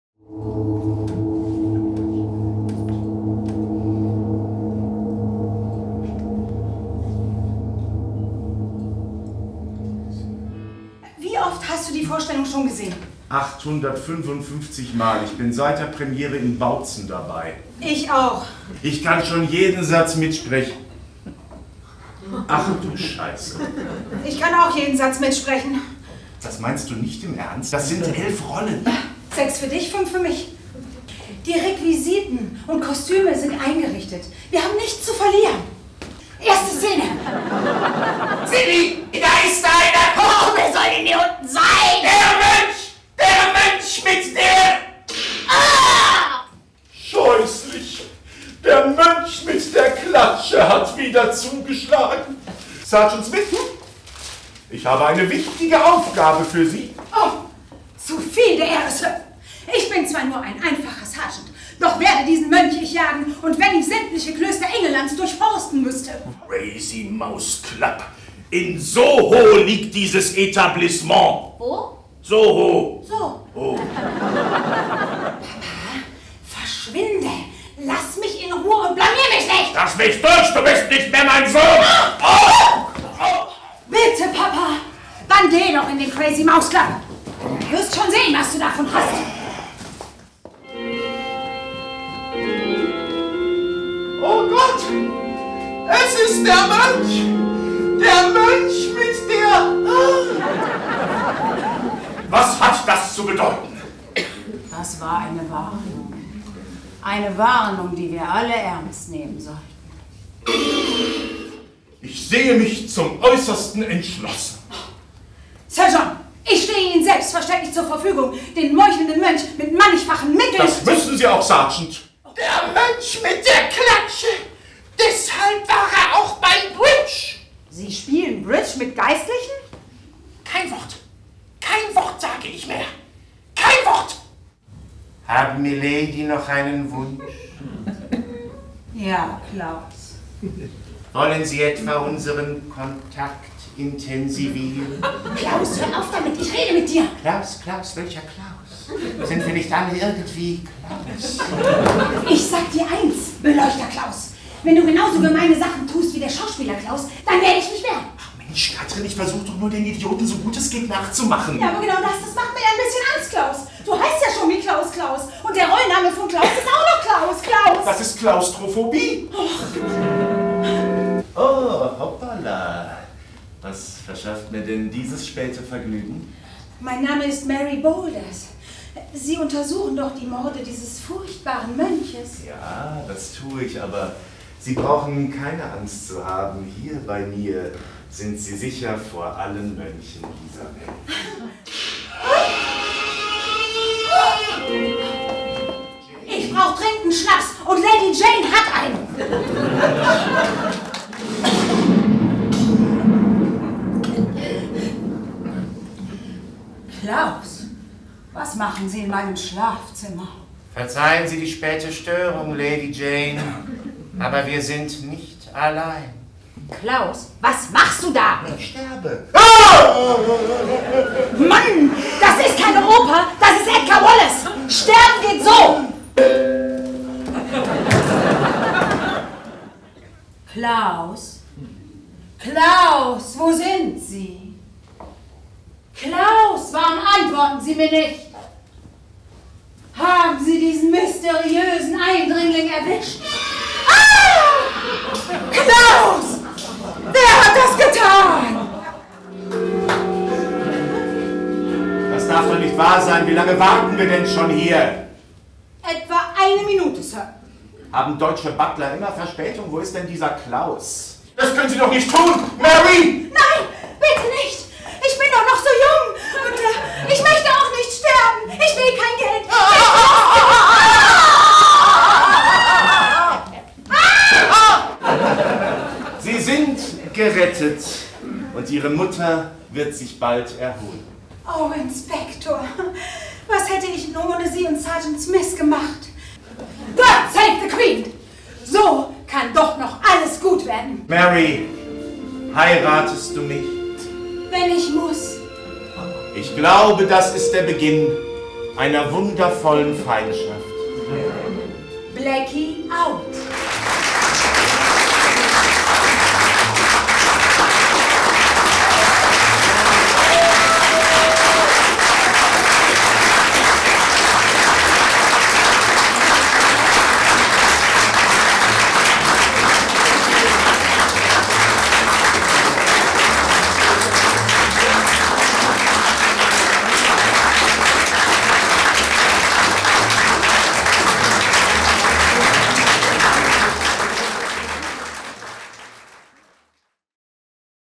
Der Mönch mit der Klatsche
Eine Katastrophe. Das Theater ist voll, aber die Schauspieler der Krimicompany stehen samt Bühnenbild im Bus auf einer gesperrten Autobahn. Es ist die 856. Vorstellung des Schockers Der Mönch mit der Klatsche. Nur die Regie-assistentin Karin Tor und der Requisiteur Klaus Konski sind im eigenen Auto vorausgefahren. Nun stehen sie vor dem Publikum. Sie sind in Geldnot und brauchen dringend die Abendgage. Sie können die Texte, sie haben sie 855 Mal gehört. Also entschließen sie sich, alle Rollen selbst zu spielen. Mit ein paar Kostümteilen und Requisiten, mit Musikeinspielungen und den paar Effekten, die sie dabeihaben. In wilder Improvisation springen sie von Rolle zu Rolle und zaubern eine abgedrehte, komödiantische Krimishow auf die Bühne, die immer wieder zu scheitern droht. Aber mit waghalsigen Wendungen schaffen die beiden das Unmögliche.

Freunde der Edgar-Wallace-Krimis erkennen die typischen Elemente. Es gibt wahnsinnige Mörder, einen skurrilen Butler, eine Jungfer in Nöten und einen liebenswerten, aber schusseligen Ermittler von Scotland Yard. Der Mönch mit der Klatsche ist eine liebevolle Erinnerung an die gruselig-charmanten Filme aus den sechziger Jahren. Und zugleich eine rasante Komödie von heute, mit zwei Darstellern, die um ihr Überleben spielen.